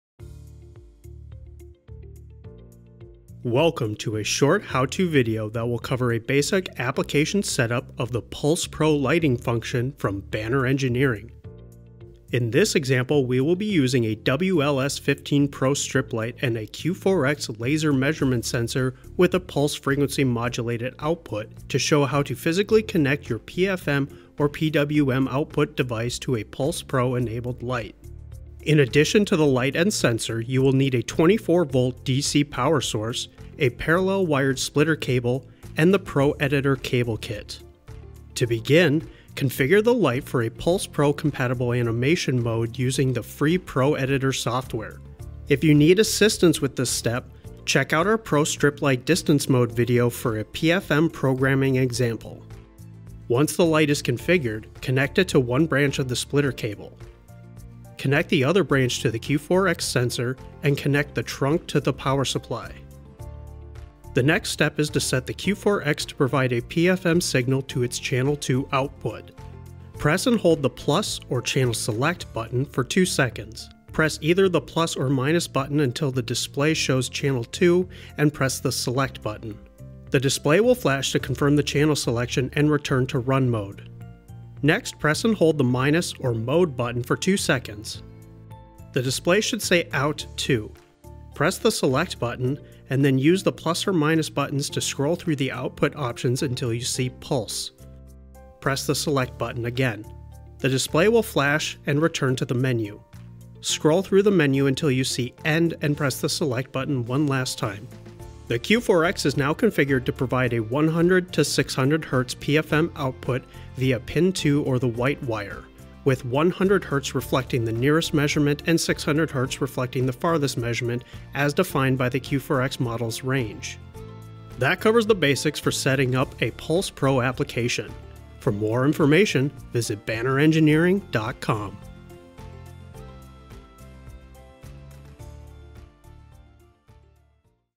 Pulse Pro Application Setup
In this video, learn how to setup a basic Pulse Pro application using a Pro Enabled Strip Light and a Q4X Laser Measurement Sensor.